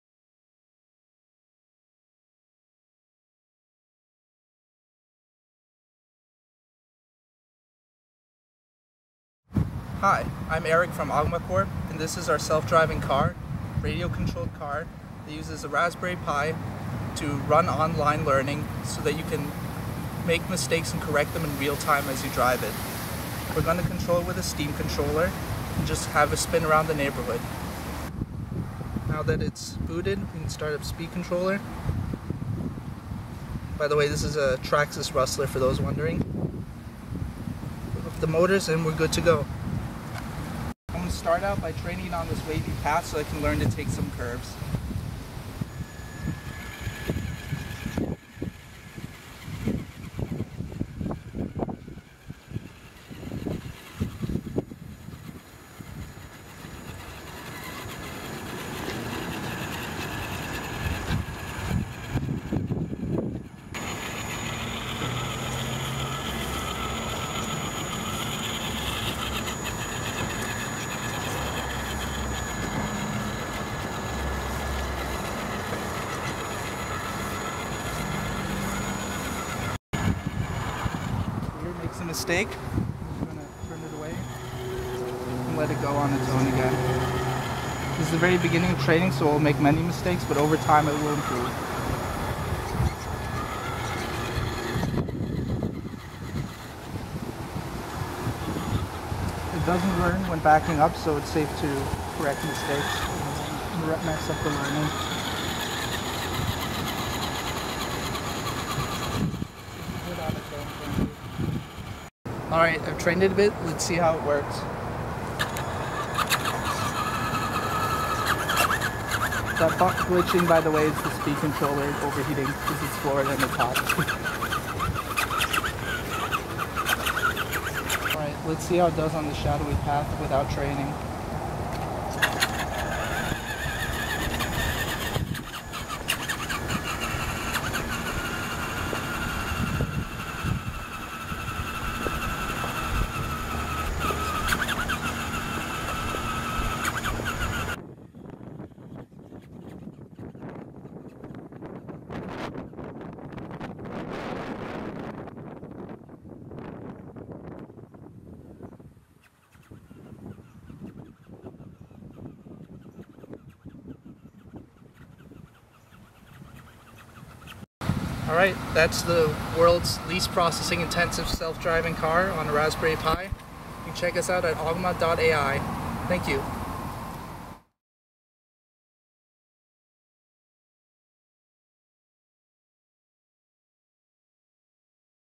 Self Driving Car Learns Online and On-board on Raspberry Pi 3
Eric Laukien, Head of R&D at Ogma demonstrates a self-driving car based on an inexpensive RC car, a Raspberry Pi 3 and an Arduino for interfacing between them. As far as we know, this is the first example of on-board, online learning to control a car from raw video and control inputs on a Raspberry Pi. The software (which will be released later this month uses the latest version of Ogma's Feynman Machine technology.